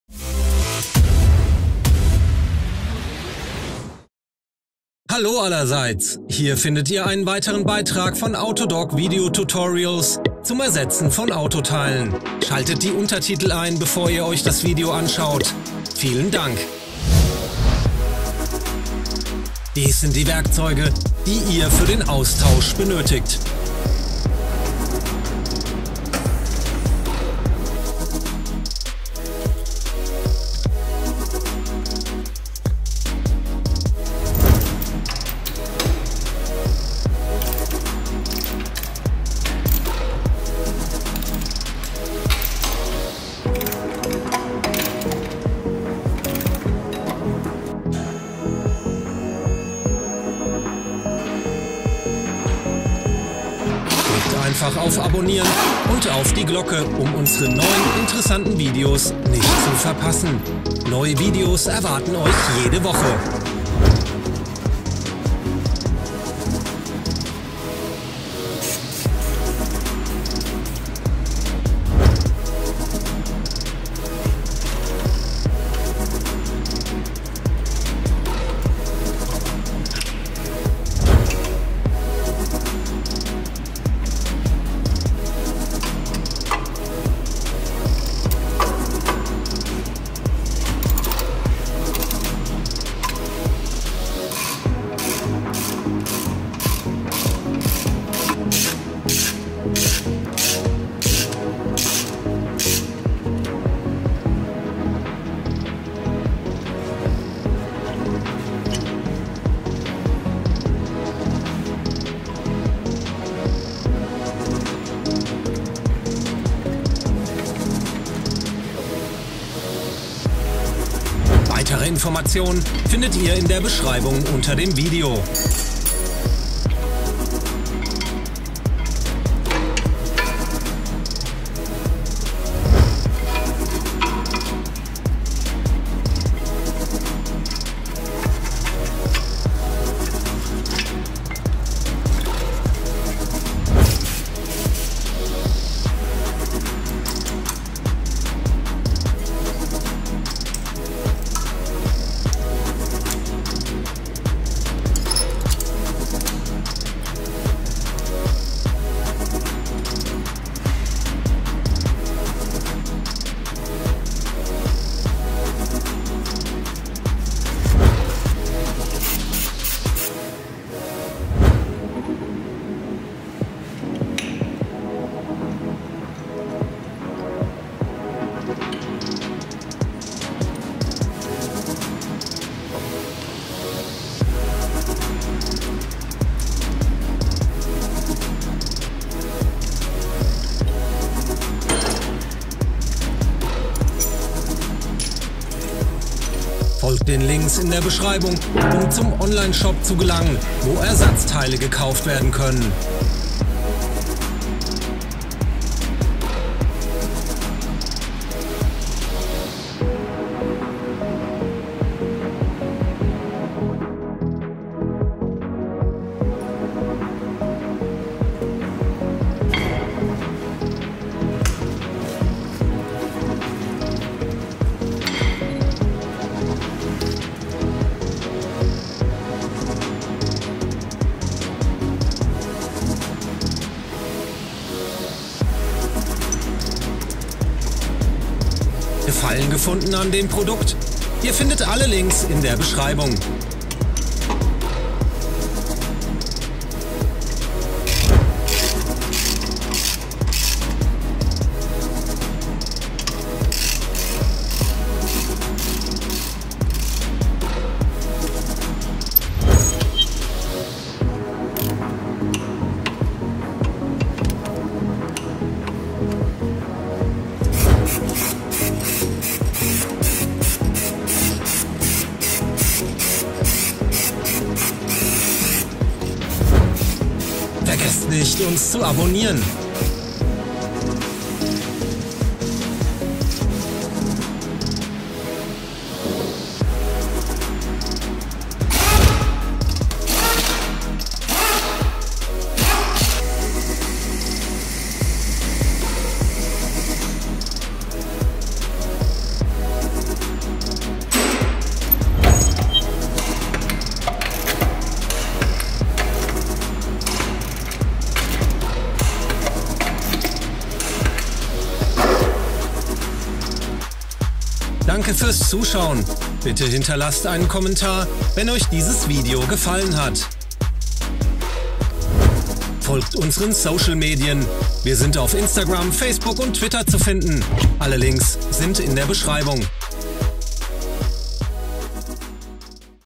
Wie TOYOTA LAND CRUISER PRADO 3 (J120) Bremsbeläge hinten / Bremsklötze hinten wechseln [AUTODOC]
Wie TOYOTA LAND CRUISER PRADO 3 (J120) 3.0 SUV 09.2002–08.2010 Bremsbeläge hinten / Bremsklötze hinten / Bremsbelagsatz hinten / Scheibenbremsbeläge hinten wechseln [AUTODOC TUTORIAL]

Bremsbeläge an TOYOTA FJ CRUISER (GSJ1_) (11.2010) / TOYOTA Land Cruiser Prado 150 (J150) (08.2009) / LEXUS GX (J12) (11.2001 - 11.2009) / LEXUS GX (11.2009) Fahrzeugen, werden stets nach demselben Muster ersetzt. Es kann jedoch aufgrund spezifischer Merkmale des Fahrzeugdesigns kleinere Unterschiede geben.

🔝 Top Marken-Hersteller für Bremsbeläge: RIDEX; STARK; BREMBO; BOSCH; ATE

🛒 Auf unserer Webseite finden Sie eine breite Palette an Autoersatzteilen, die für Sie interessant sind

🛠️ Benötigte Werkzeuge, um Bremsbeläge hinten zu ersetzen:
- Drahtbürste
- Bremsenreiniger
- Kupferpaste
- Anti-Quietsch-Paste
- Impact-nuss für felgen nr. 21
- Drehmomentschlüssel
- Flacher schraubendreher
- Splinttreiber
- Gummihammer
- Brechstange
- Unterlegkeil